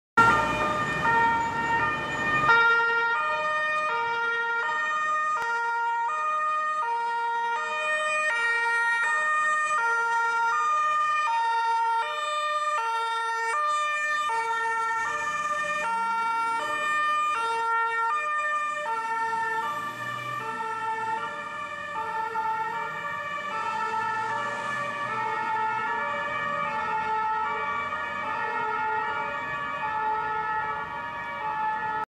LF 16/12 + DLK 23/12 Fw3
die Löschgruppe der Fw3 in Nürnberg.
the laddertruck and the engine responding with lights and siren in nuremberg.
Im Winter 2010